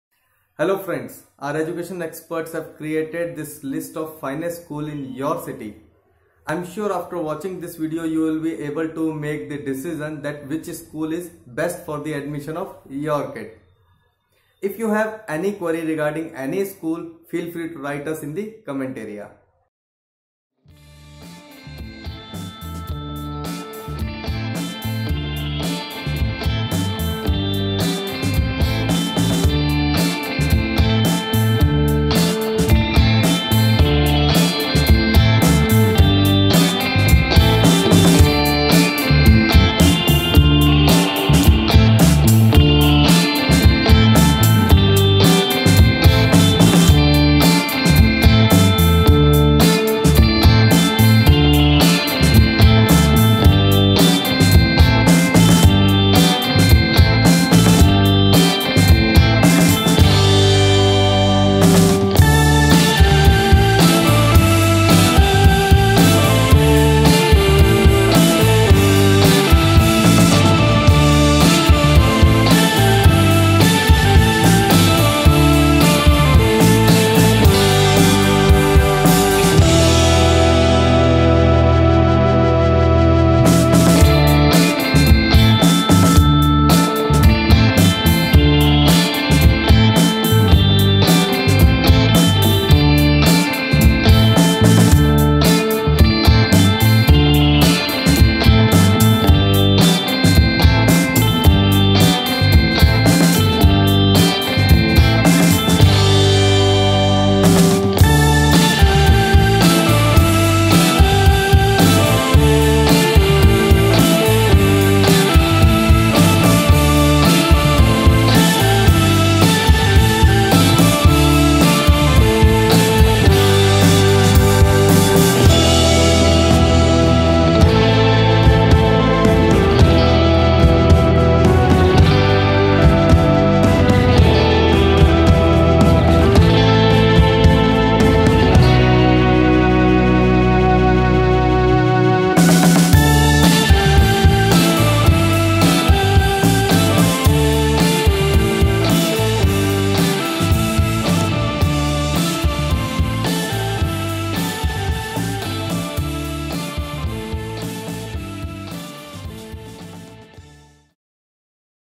Schools around Leighton Buzzard, United Kingdom
Must Watch.. Best Schools around Leighton Buzzard

Click on Bell ICON to get the notification of New Videos.This is not rank wise listing of Top Schools in Leighton Buzzard, it's the list of best Schools around Leighton Buzzard as per the ratings given by our users.

Listed Schools:

Leighton Middle School
Church Square

Leedon Lower School
Leighton Buzzard, United Kingdom

Southcott Lower School
Bideford Green

The Rushmere Park Academy
East St

Oak Bank School
Sandy Lane